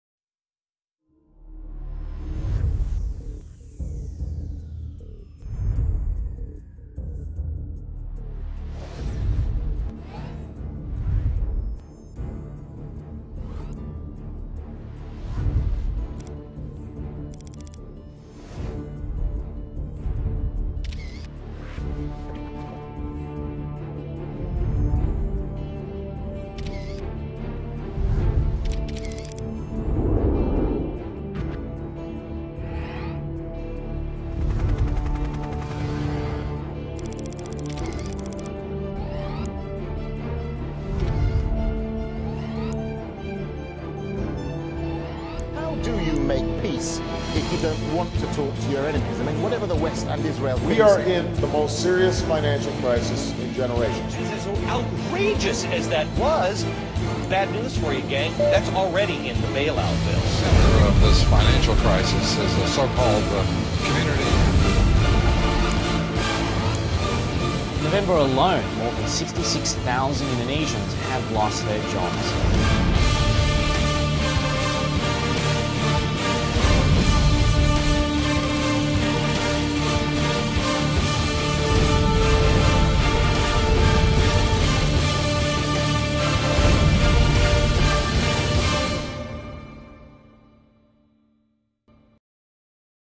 Farhat demo Reel Videographer / editor
This is a demo reel for Farhat Abbas. A graduate of International academy of design and technology (Toronto Film School). Farhat is videographer and editor.